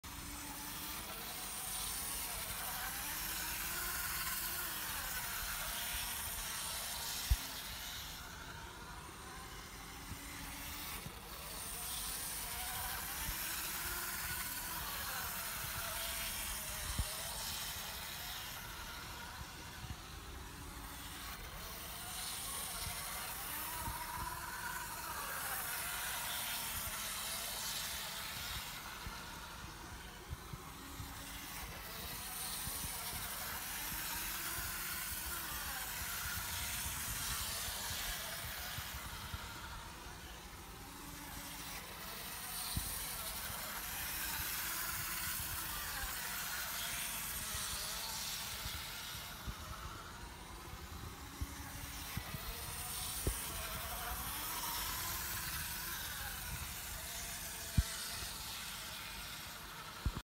My Märklin 8800 BR 89 short rolling
Due to my main track layout is still under renovation, i warming up my locos on this small oval track. My BR 89 is particularly “fond” with.